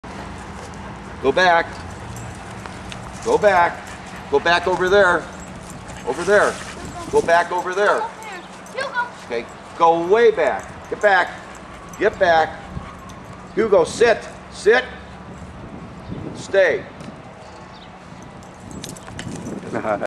Move back Smart Dog
Neighbors dog playing fetch.